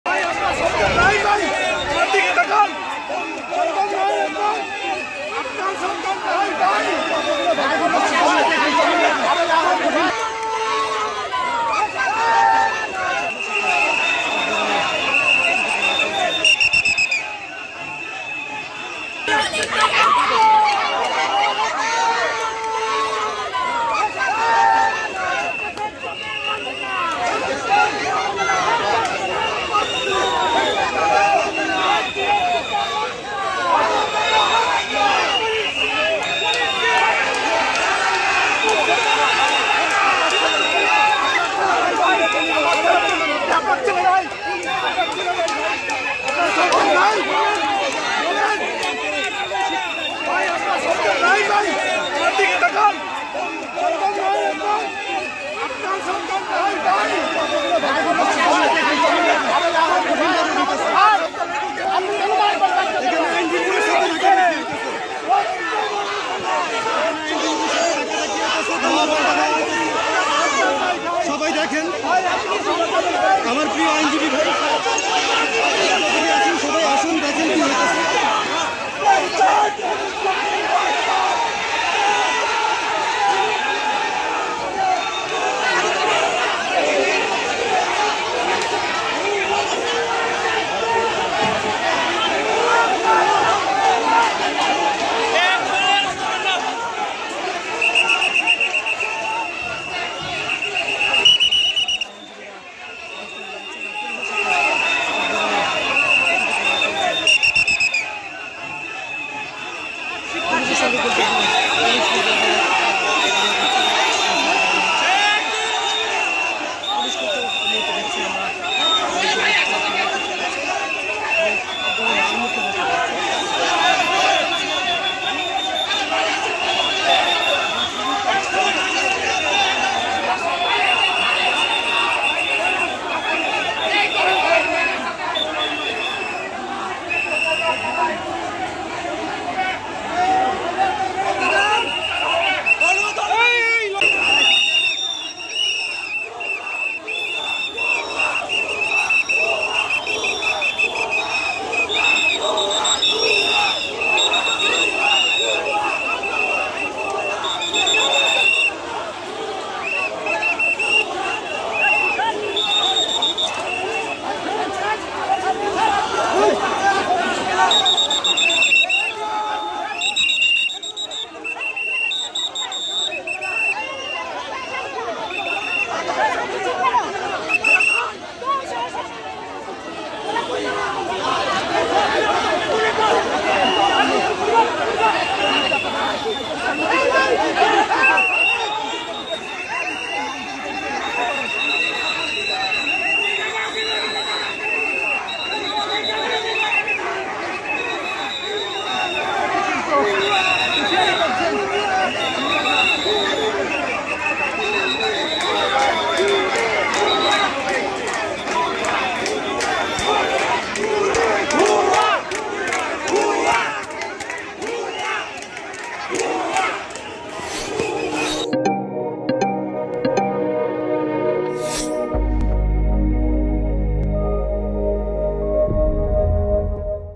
হাইকোর্টের সামনে শিক্ষার্থী-আইনজীবীদের সঙ্গে পুলিশের ধস্তাধস্তি | Manzur Al Matin | Samakal News
হাইকোর্টের সামনে শিক্ষার্থী-আইনজীবীদের সঙ্গে পুলিশের ধস্তাধস্তি | Lawyer Manzur Al Matin | Samakal News